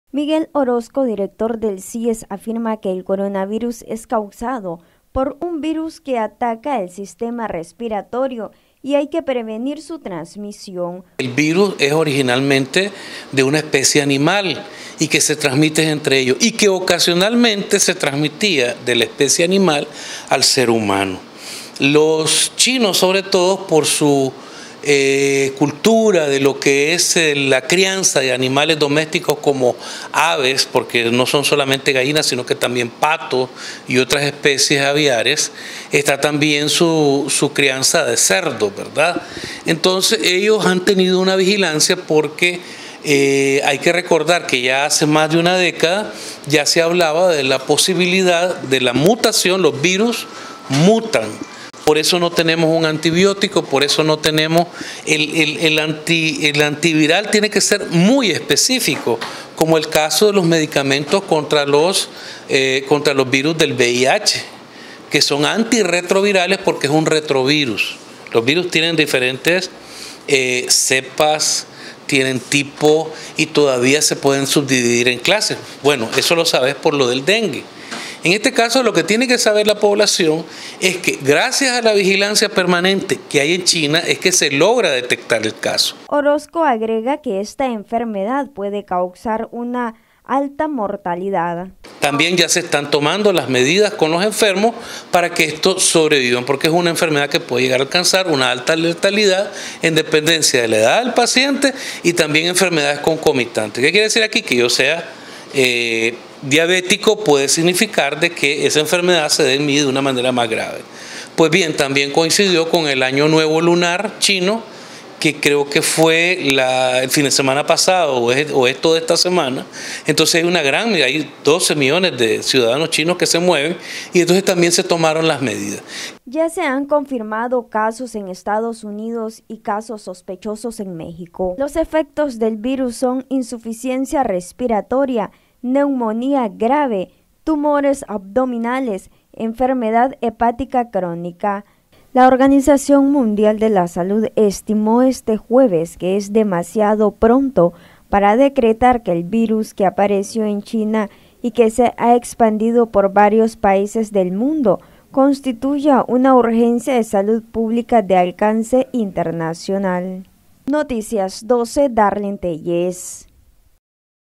OROZCO: “CORONAVIRUS ATACA EL SISTEMA RESPIRATORIO Y HAY QUE PREVENIR SU TRANSMISIÓN”
Director del CIES  Miguel Orozco,  afirma que  Coronavirus ataca el sistema respiratorio y  hay que prevenir su transmisión.